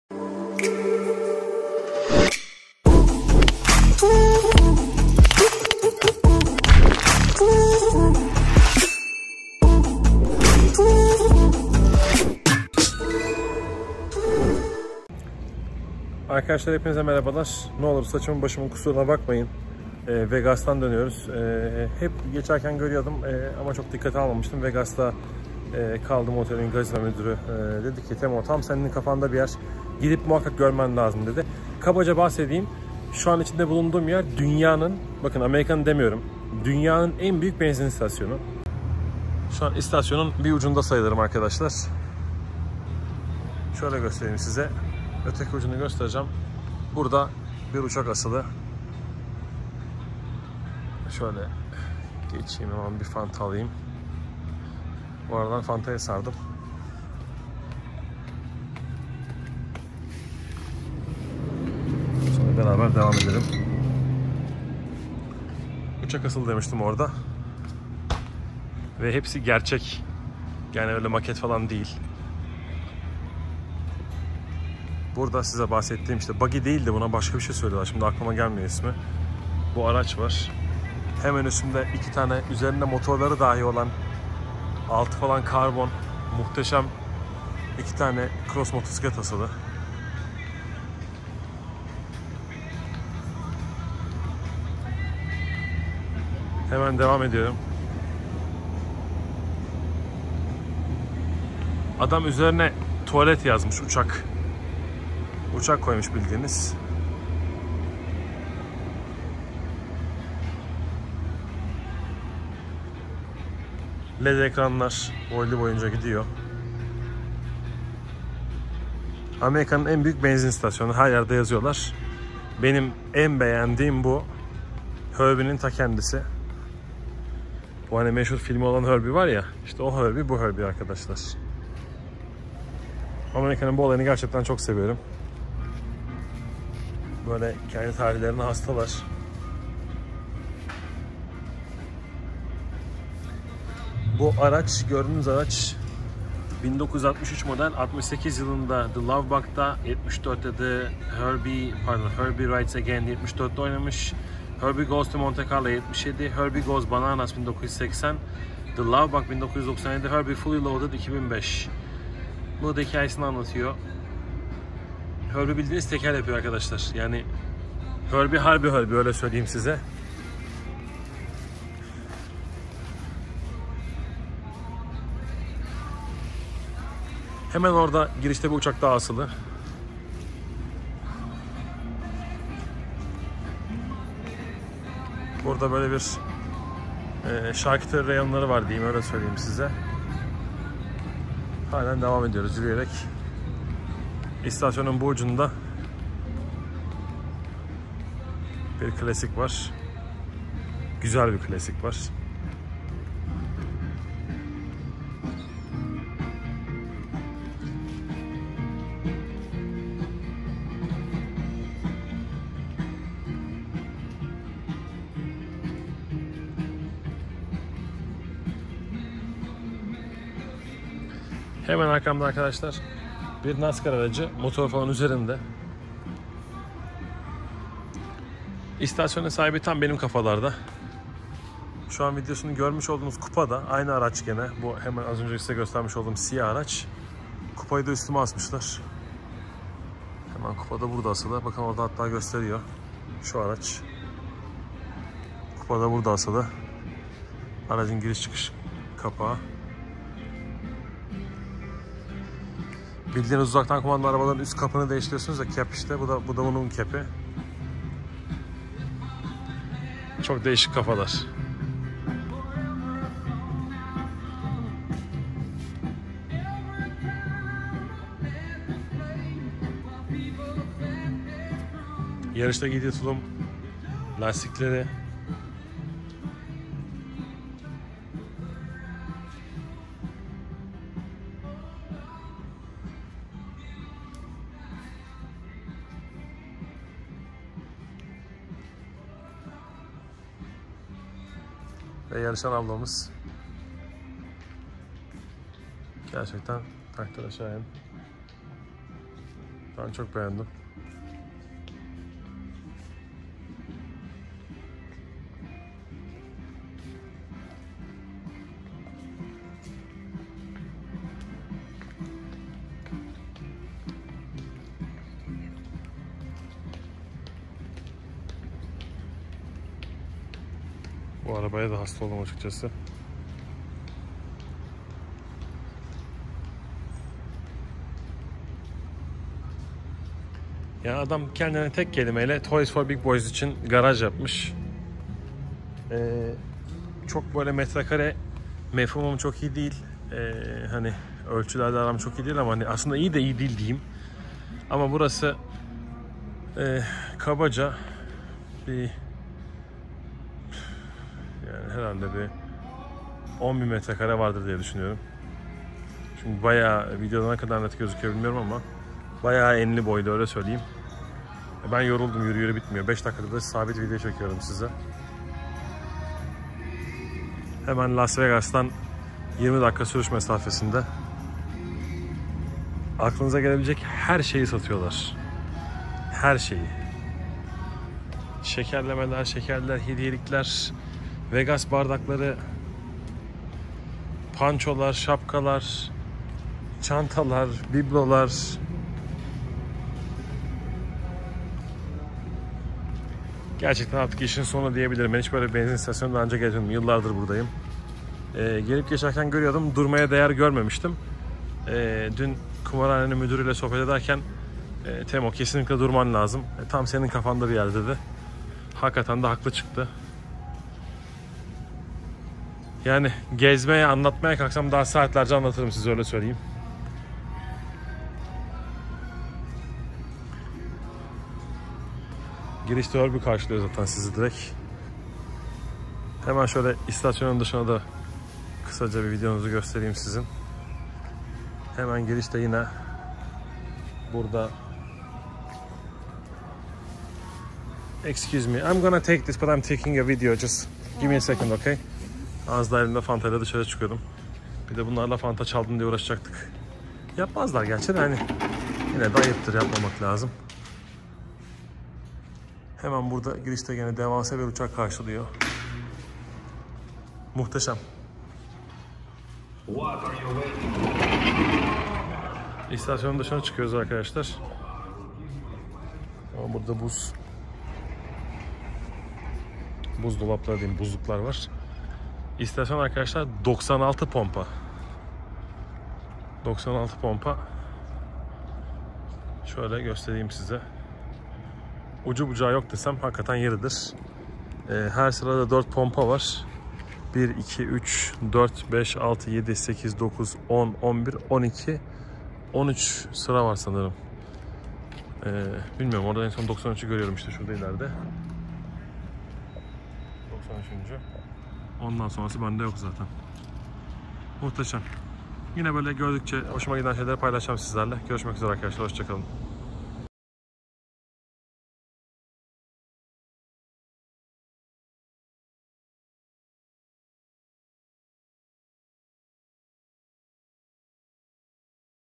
DUNYANIN EN BUYUK BENZIN ISTASYONU
Selamlar Arkadaslar ,

Las Vegastan California ya giderken hep yolun uzerinde goruyordum tabelasini ama hic durmaya deger gormemistim bu sefer Cosmopolitan otelin casino mudurunun tavsiyesi uzerine durdum ve pas gectigim her sefer icin pisman oldum elimden geldigince sizlere de gostermeye gayret ettim ancak tek kelime ile buyuleyici muhtesem bir benzin istasyonu hic aklima gelmezdi dunyanin en buyuk benzin istasyonunun bu kadar ilgimi cekebilecegini iceride tabiri caiz ise YOK YOK !!! SONUNA KADAR IZLEYIN HER KOSESI AYRI KEYIFLI BIR ISTASYON OLMUS IYI SEYIRLER !!!